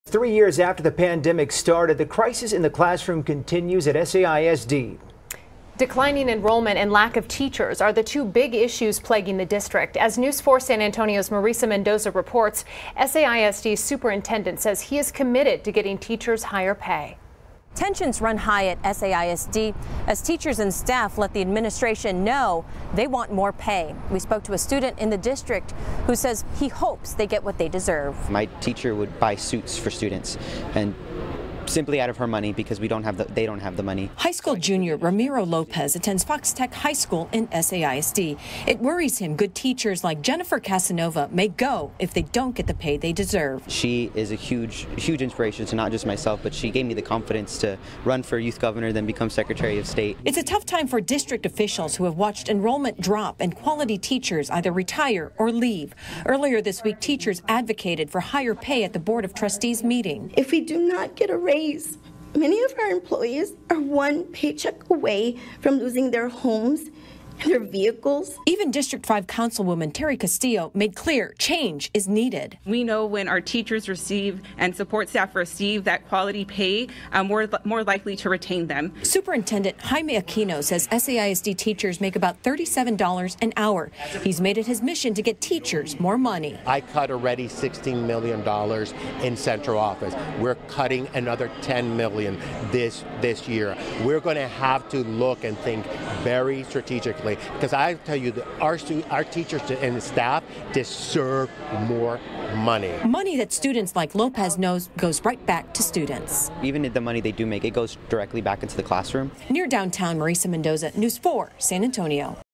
San Antonio ISD teachers demand a livable wage
It’s a tough time for district officials who have watched enrollment drop and quality teachers either retire or leave. Earlier this week, teachers advocated for higher pay at the board of trustees meeting.